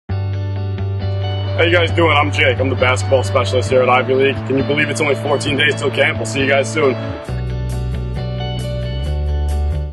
Countdown to Day Camp - Day 14 - Jake
Only 14 days left! Jake just can't wait to get on the court with our campers.